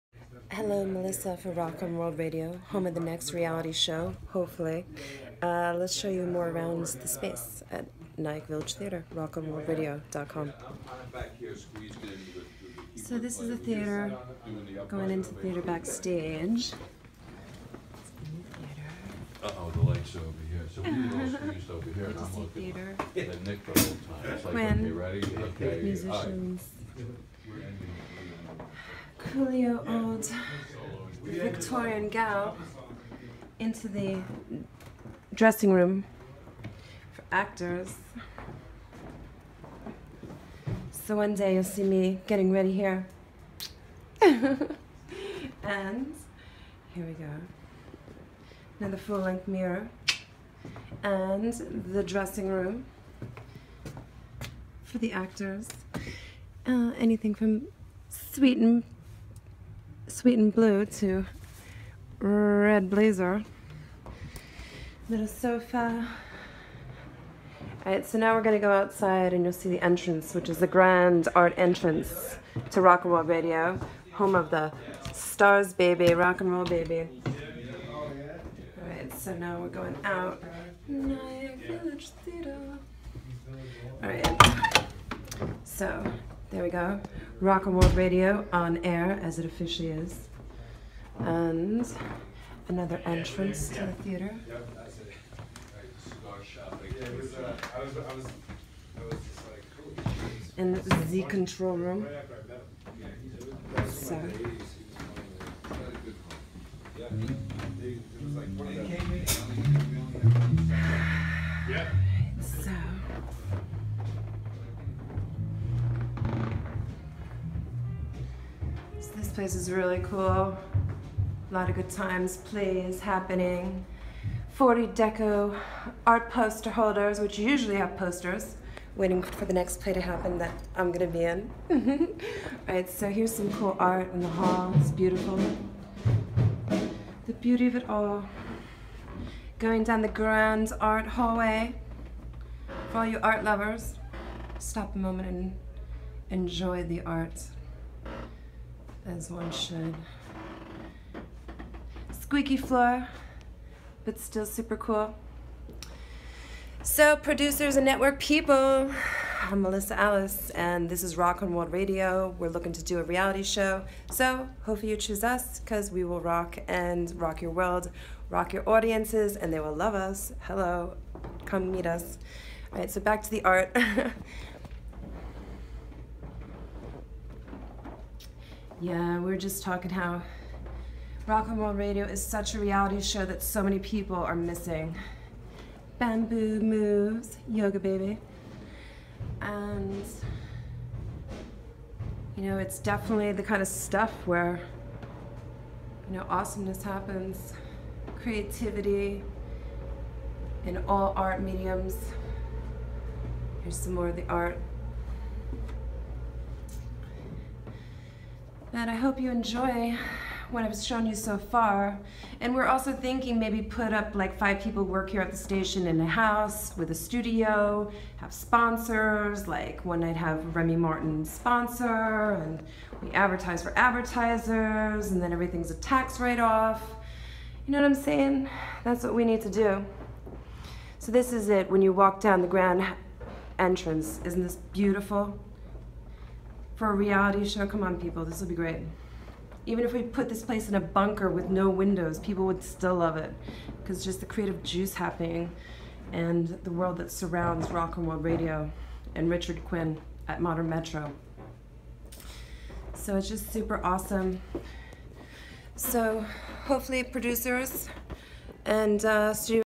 Reality show idea promo
Tour of the Nyack village theatre home of Rockland World Radio Promo video too pitch a reality tv show surrounding the internet radio station.